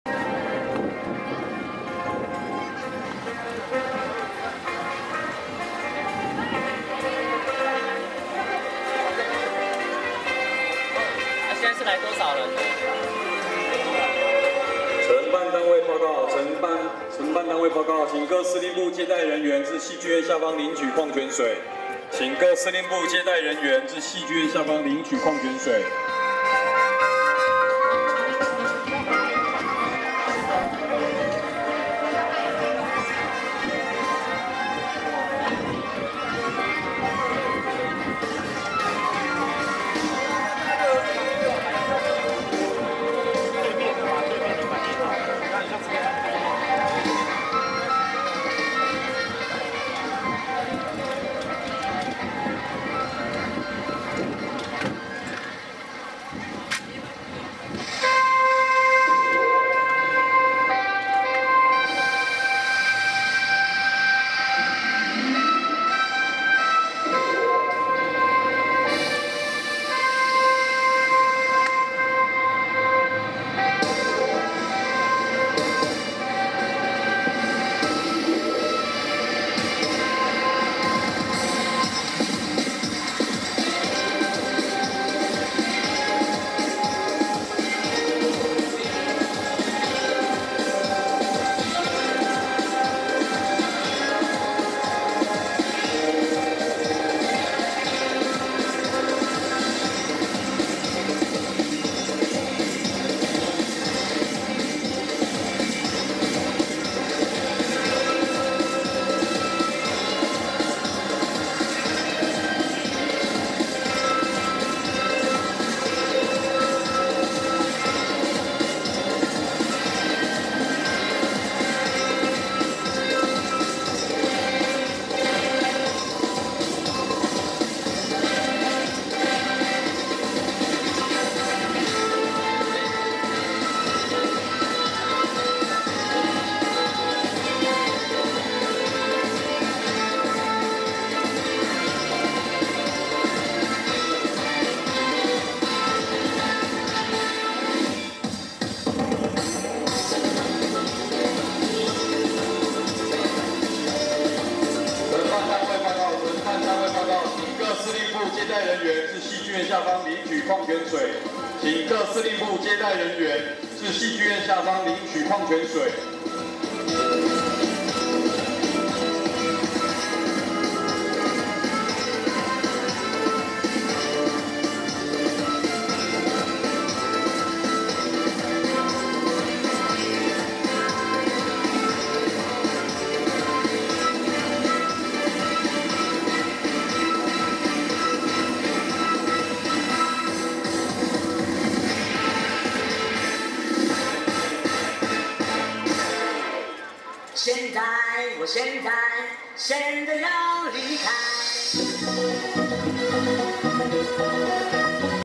20110529 孫自佑在台北軍樂展彩排進場畫面
20110529 孫自佑在台北軍樂展彩排進場畫面